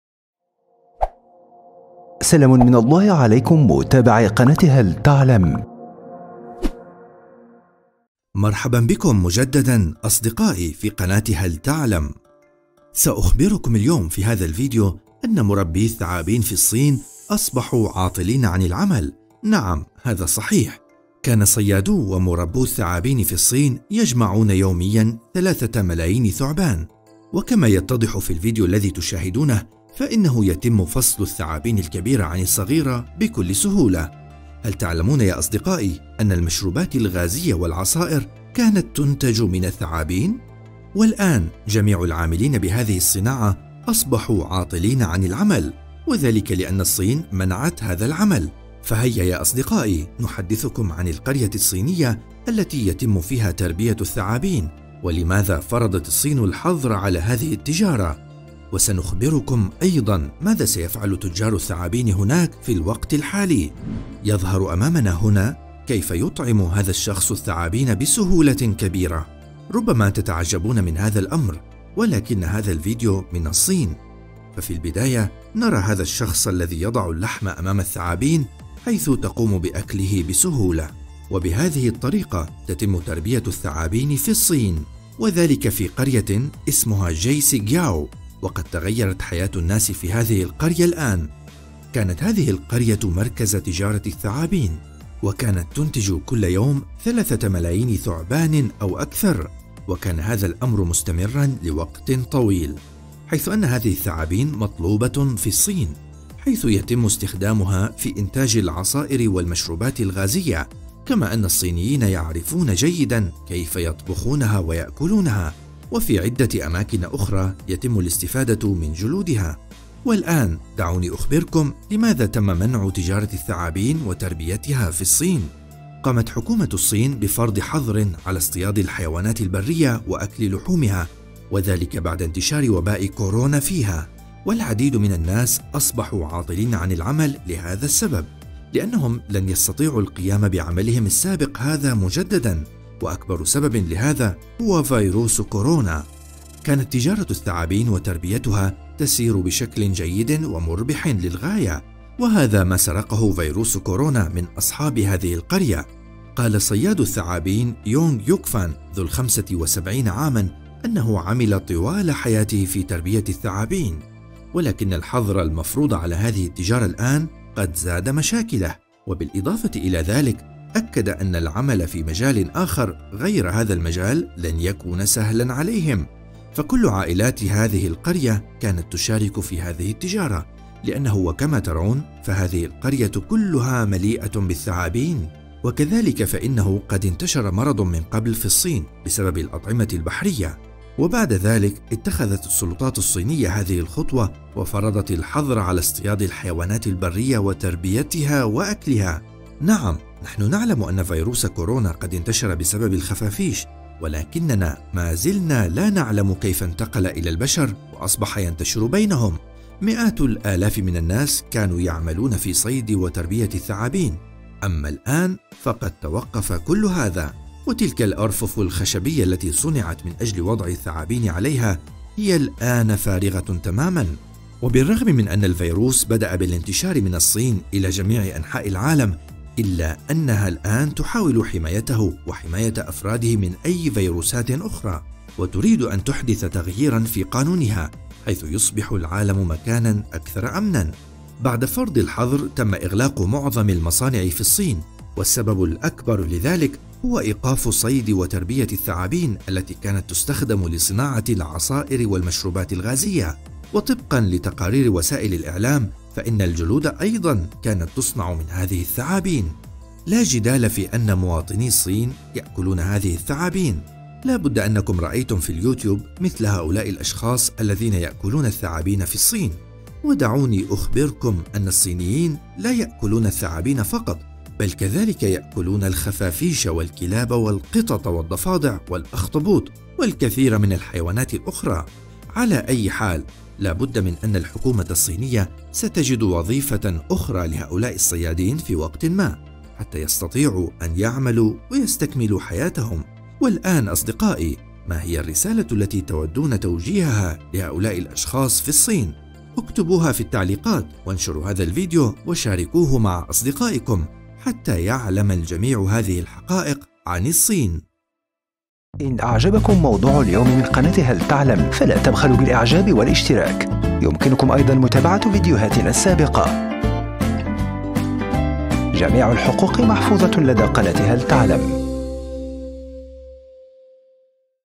شاهد ما حدث لمربيّ الثعابين في الصين.. ممنوع لاصحاب القلوب الضعيفة !!!
سأخبركم اليوم في هذا الفيديو أن مربيّ الثعابين في الصين أصبحوا عاطلين عن العمل ، نعم .. هذا صحيح .. كان صيادي ومربي الثعابين في الصين يجمعون يوميًا 3 ملايين ثعبان .. وكما يتضح في الفيديو أمامكم فإنه يتم فصل الثعابين الكبيرة عن الصغيرة بكل سهولة ، هل تعلمون يا أصدقائي أنه كان يتم إنتاج المشروبات الغازية والعصائر من الثعابين !؟...

- Google Play ‏ || التطبيق الرسمي لقناة هل تعلم؟